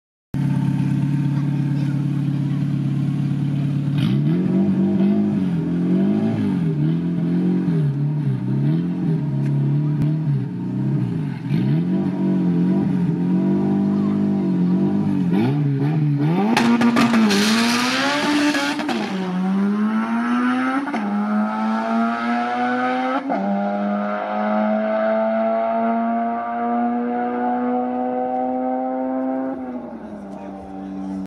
September 2019 RWYB Run 45
14th September 2019

Run What You Bring Event. Two weeks prior Round 4 of the MDRA Championship.

See you in two weeks for the September Quarter Mile Event 27-29th